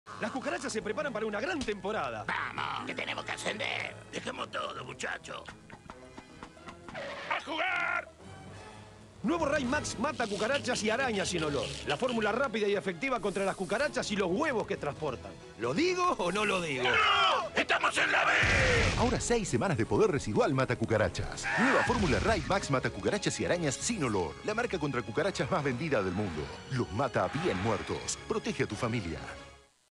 NO ESTAMOS EN LA B (Propaganda Completa de Raid Max)
Publicidad Raid Max Mata Cucarachas y Arañas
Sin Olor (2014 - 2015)
SC Johnson Argentina